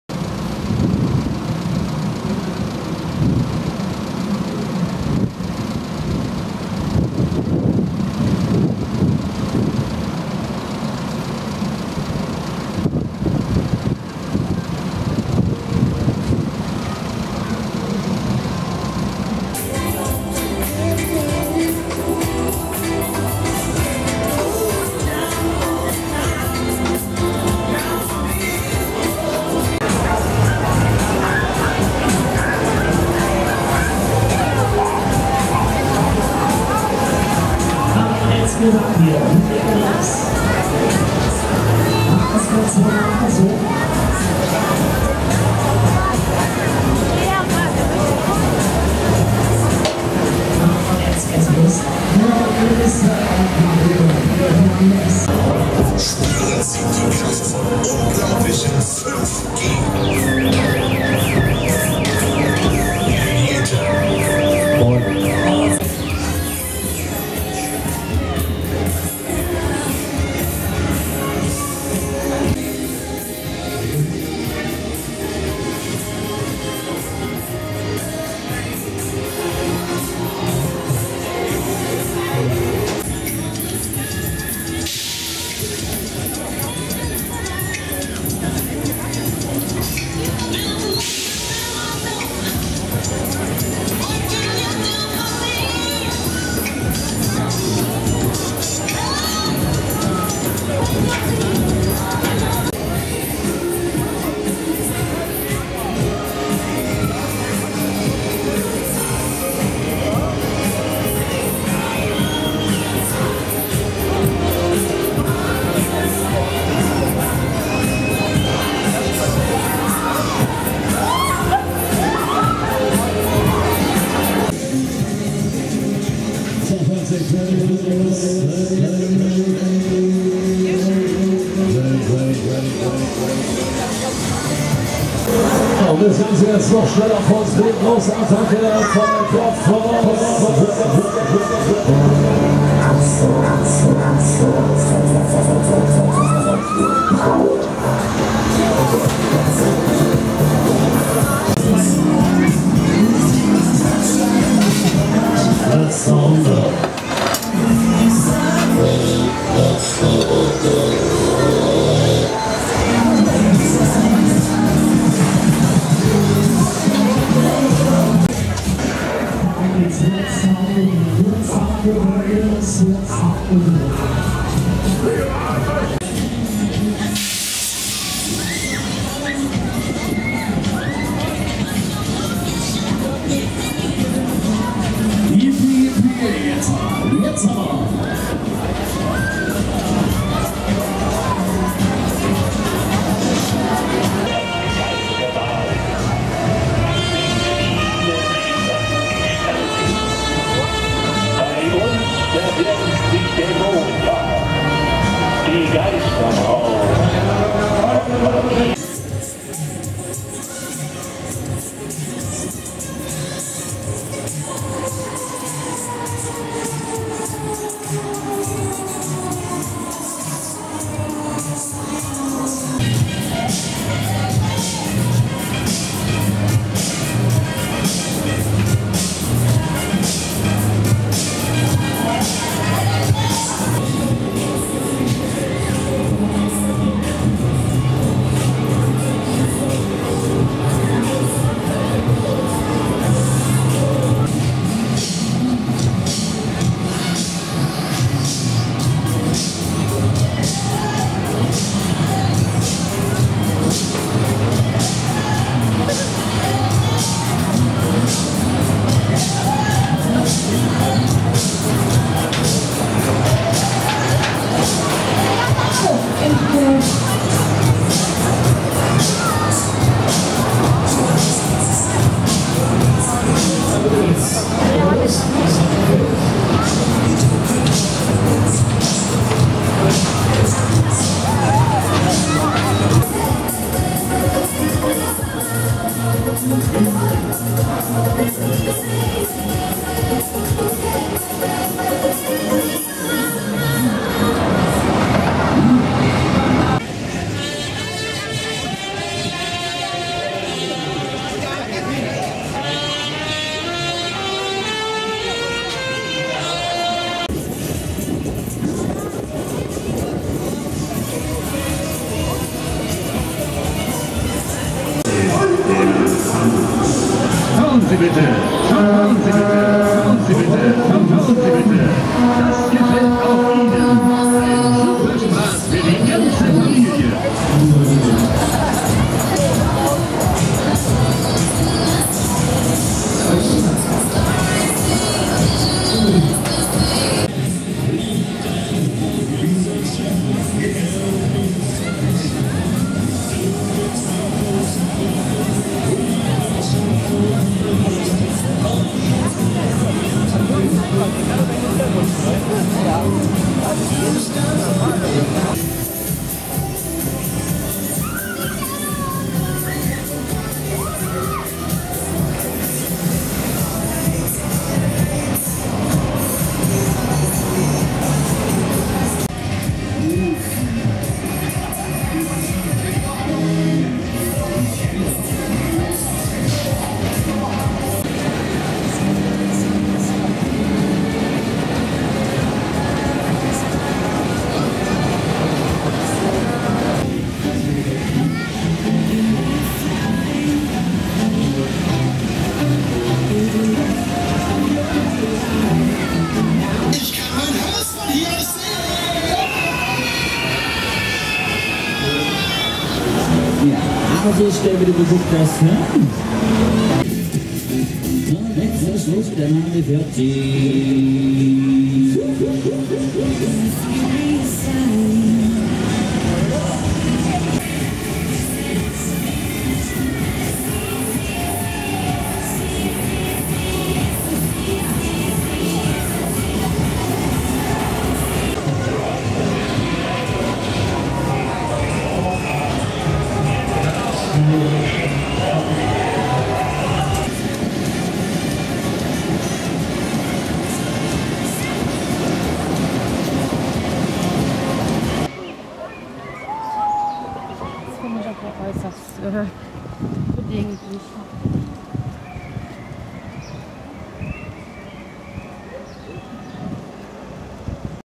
Annakirmes Düren 2013 - Die Highlights im Videomix
aufgenommen am frühen Nachmittag vor dem großen Andrang.